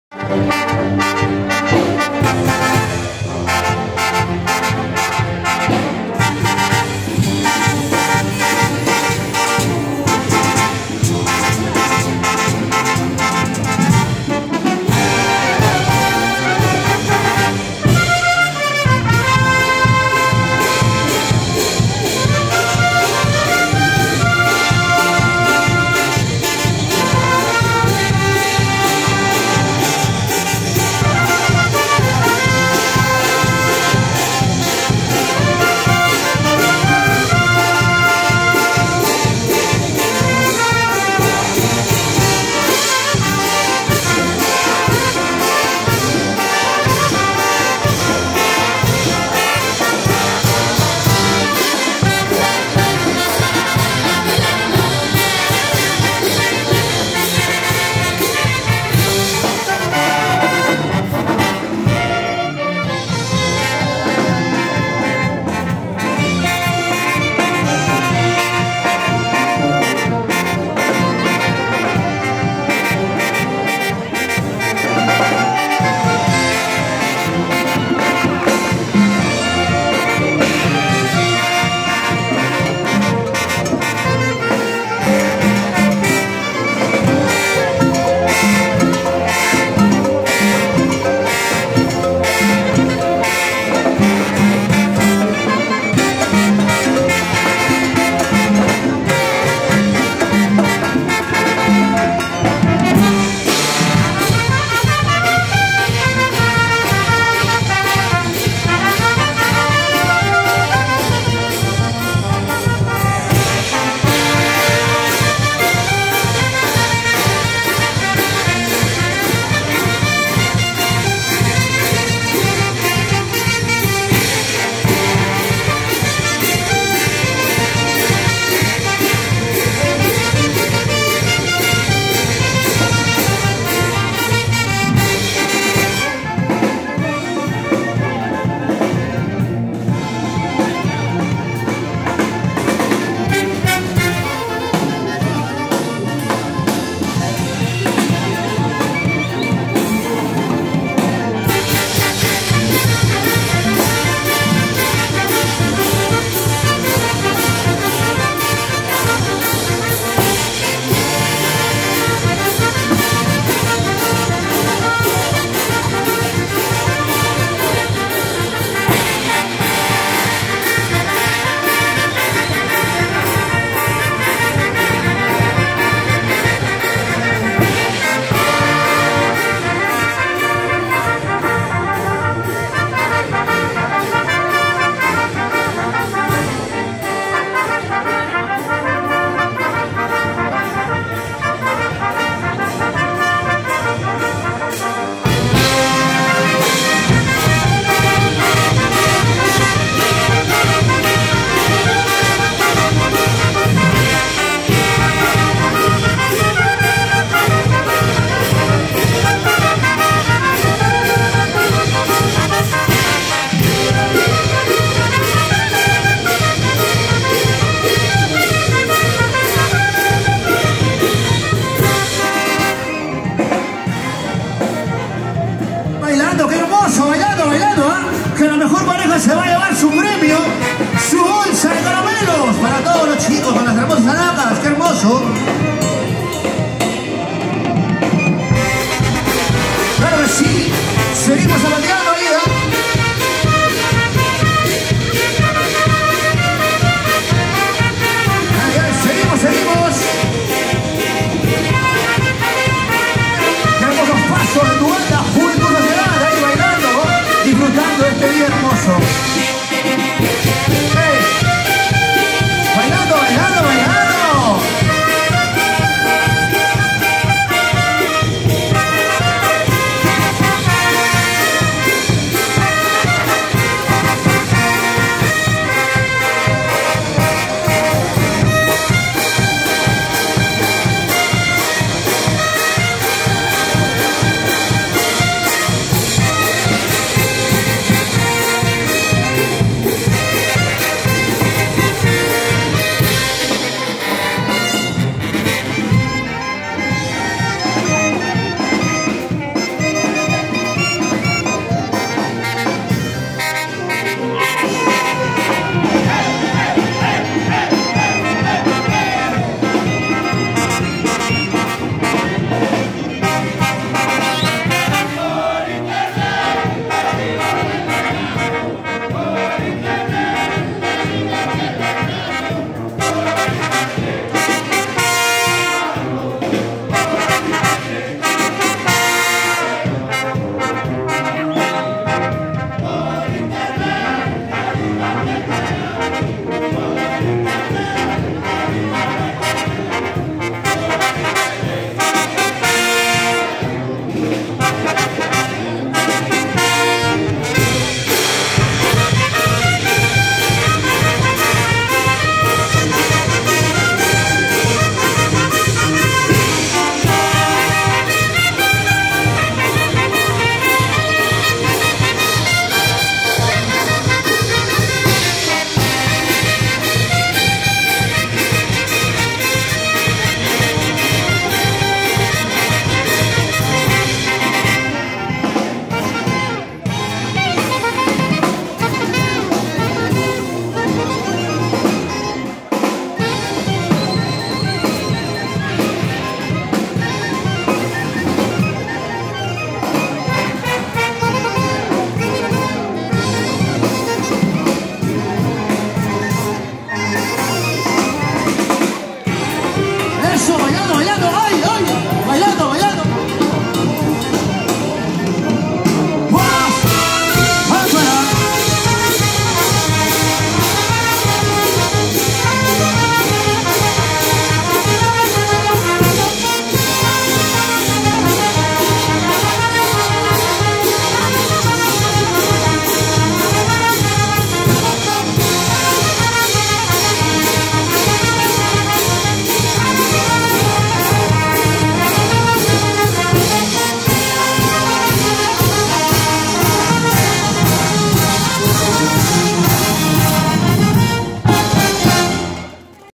BANDA SHOW JUVENTUD SOCIEDAD DE LIMA EN SU ANIVERSARIO AMIR POR INTERNET 🎺🎺🎷🎷🎷
BANDA SHOW JUVENTUD SOCIEDAD DE LIMA 17 ANIVERSARIO  EN EL LOCAL LOS ANDES
DIRECTOR.
RUBEN GAMARRA NAVEROS 🎷🎷🎷🎷🎷🎷🎷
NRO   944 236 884